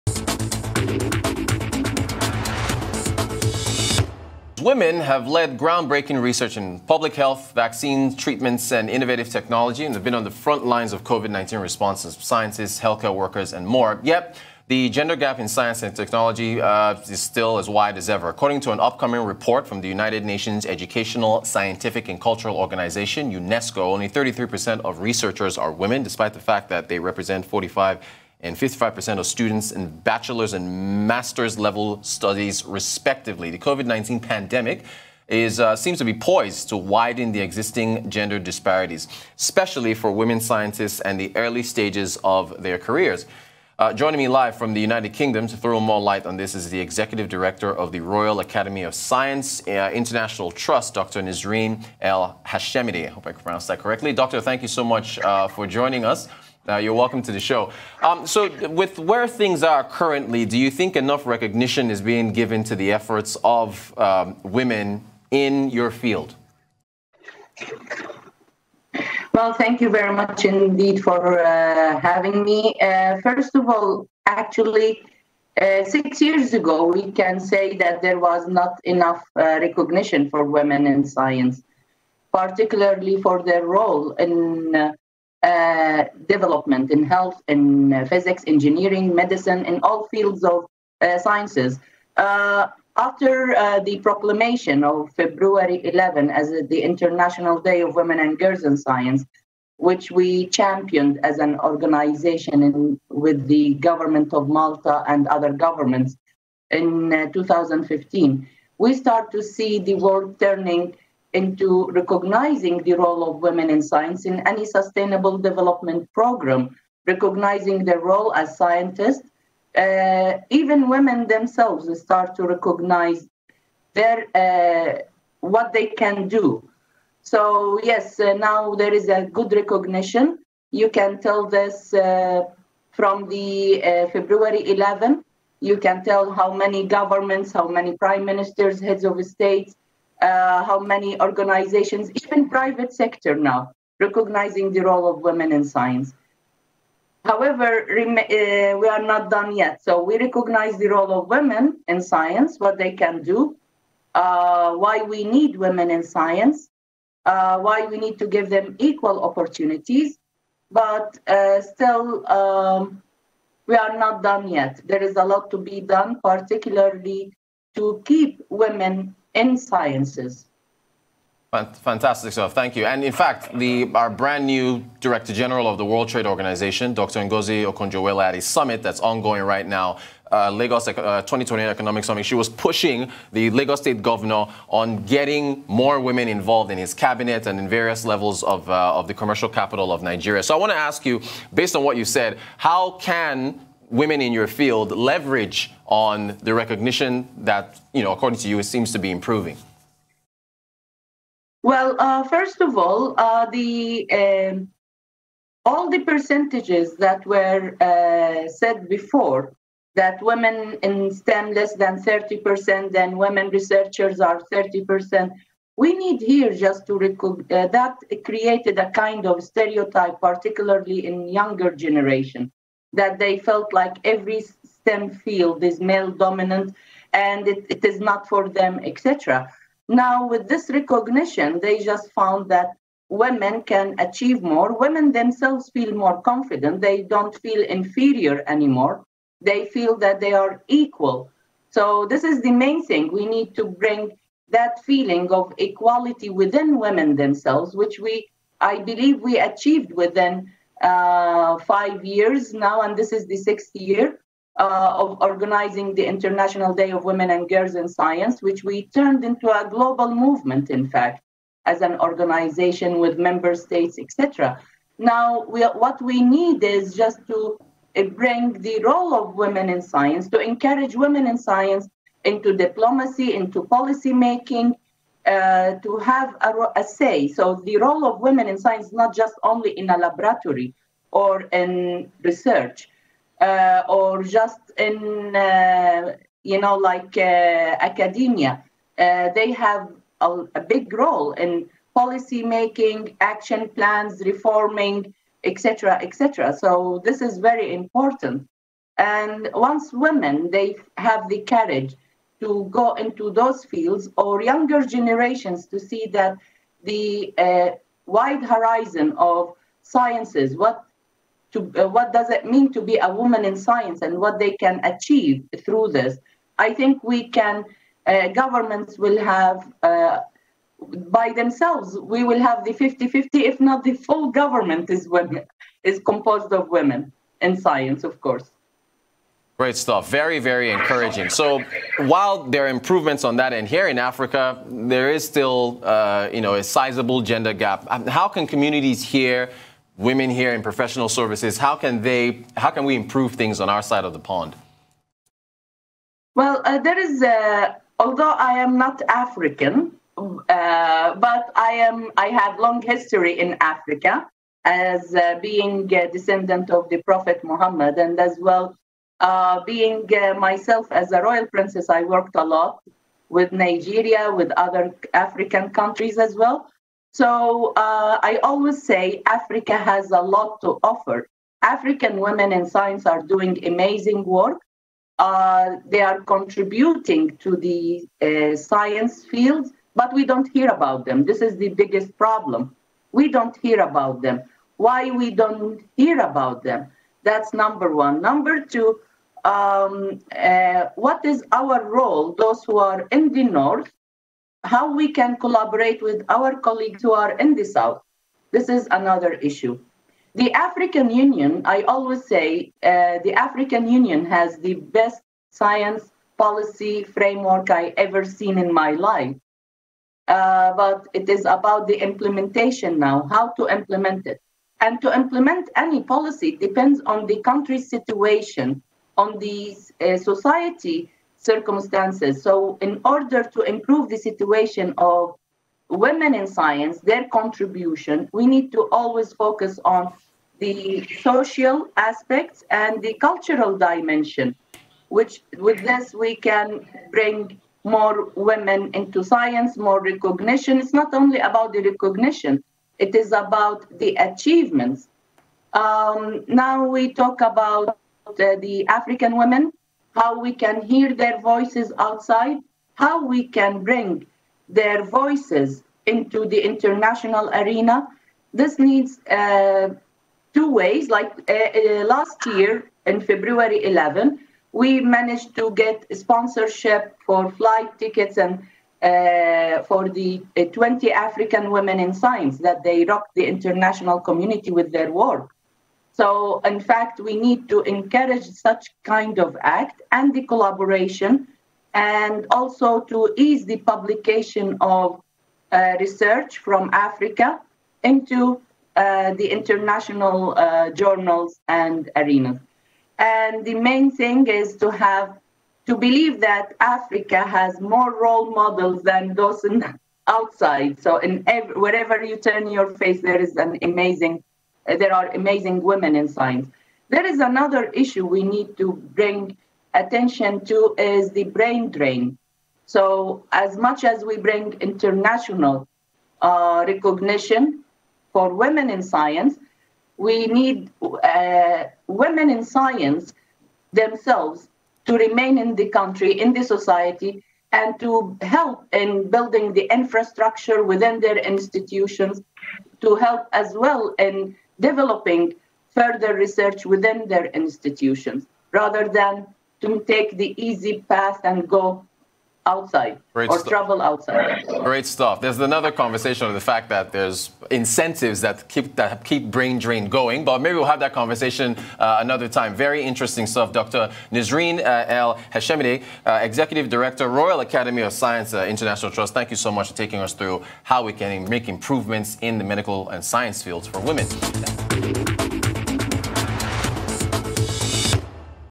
ATTRACTING YOUNG WOMEN INTO SCIENCE AND TECH - GLOBAL BUSINESS REPORT
NISREEN EL HASHEMITE, EX ROYAL ACADEMY OF SCIENCE INT'L